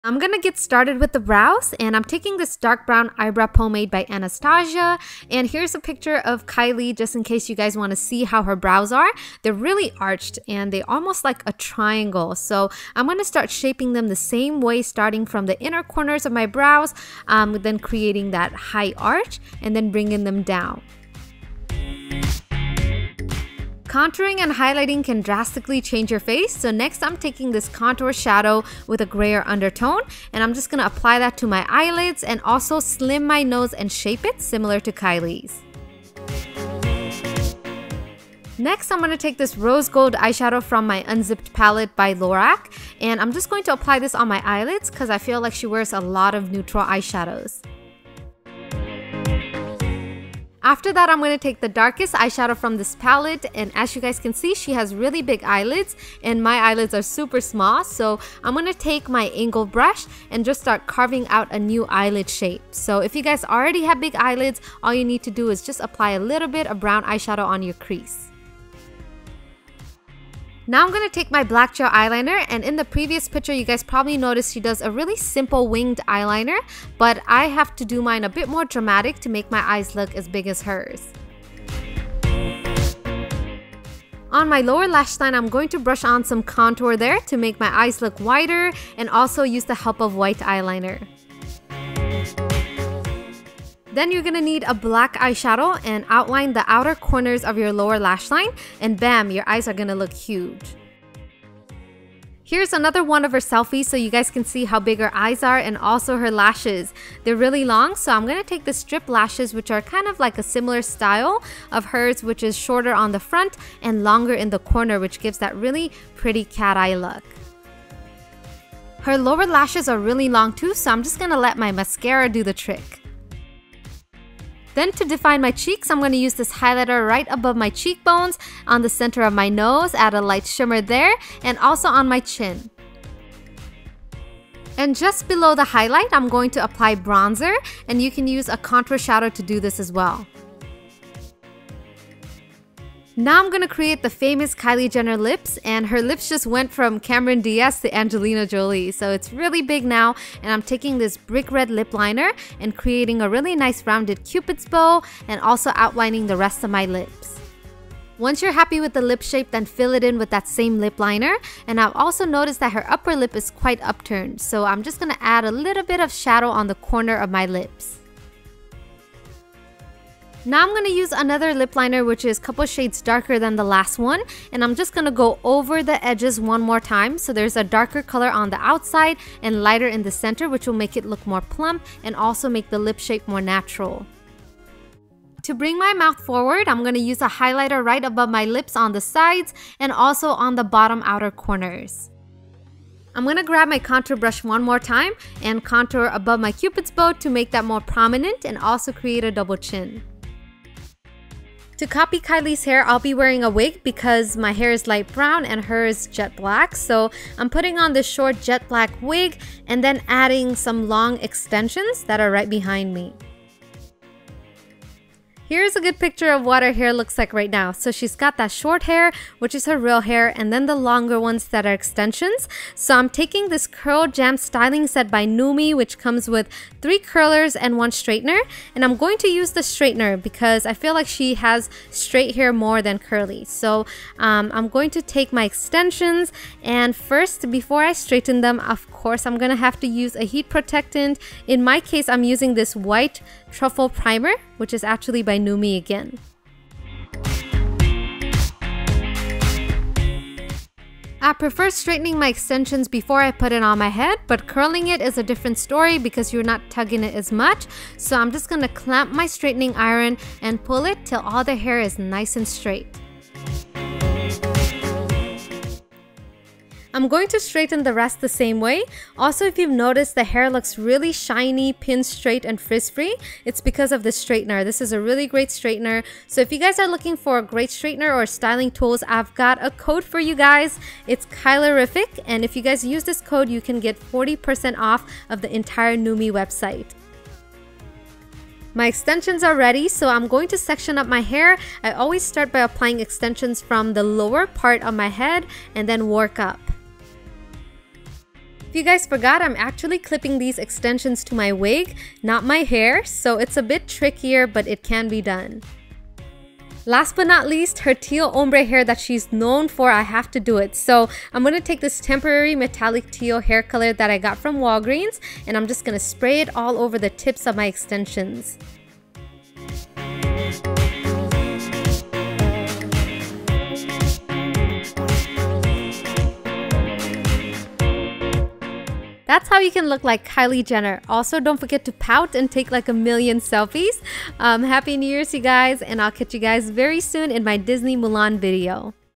How to look like Kylie Jenner !!!
USE CODE: Kylierific
  *not including Lustrum Set

Kylie Jenner look is really popular at the moment so I decided to do video on it for you guys. She's got those huge lips, arched brows and doll like eyes which was fun to recreate.
Also If an asian girl like me can look like Kylie, you guys can too and pull out your makeup bag and paint your face and take bunch of selfless and send it to me. With Makeup anything is possible

Hair Products : Nume's Curl Jam Styling set
Hair Primer : Nume's White Truffle

Makeup Products Used :
Anastasia Beverly Hills Brow Pomade 'Chocolate'
Anastasia's Contour palette
Lorac Unzipped Palette
Nyx Black Gel Eyeliner
Lancome Water Liner
Kiss I-envy False lashes KPE34
Bh Cosmetics Lilac Blush Duo
Too Faced Cosmetics Milk Chocolate Bronzer
Benefit They're Real Mascara
Mac Lip Liner 'Chicory'
UD Lip liner 'Liar'

Hair Extensions and Wig : Wig store nearby
Metallic Teal Temporary Hair color : Chroma Lights